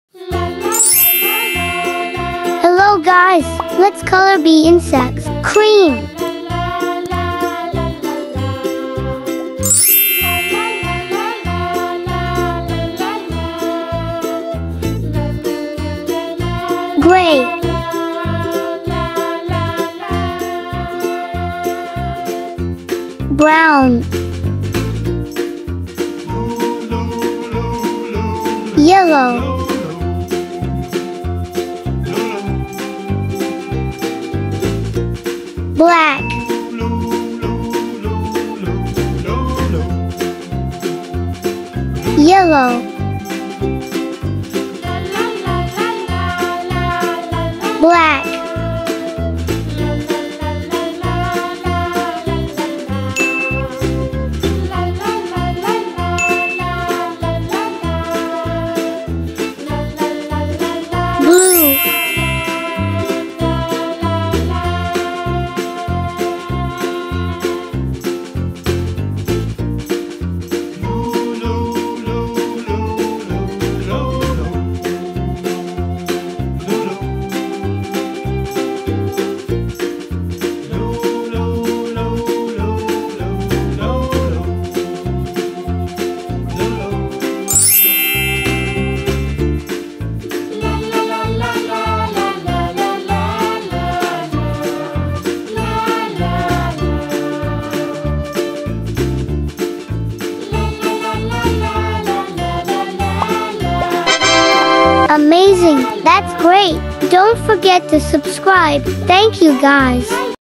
Lebah yang lucu, mewarnai gambar animasi anak-anak dan balita
hallo semuanya, ayo kita mewarnai sebuah gambar serangga lebah animasi anak-anak dan balita